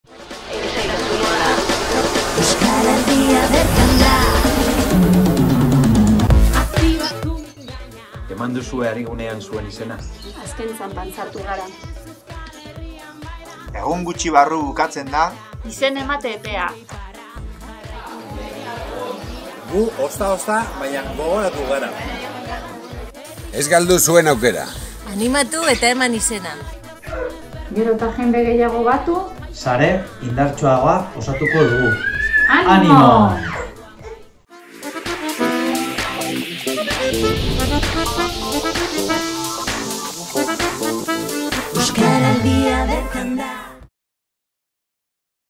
Badator Euskaraldia III
Euskaraldiko ariguneetarako izena emateko epea laster amaituko da. Oraindik izena emateko epez zabiltzate!